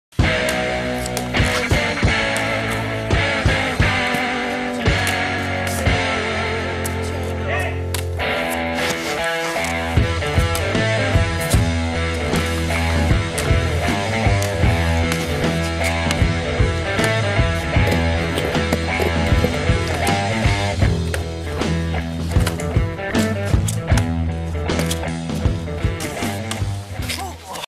Turf Snowboarding Teaser #2
Riders: John and Alec
FIlmer: Tyler and Alec
Editing: John and Tyler
Camera: Canon Powershot SX30 IS and GoPro HD Hero2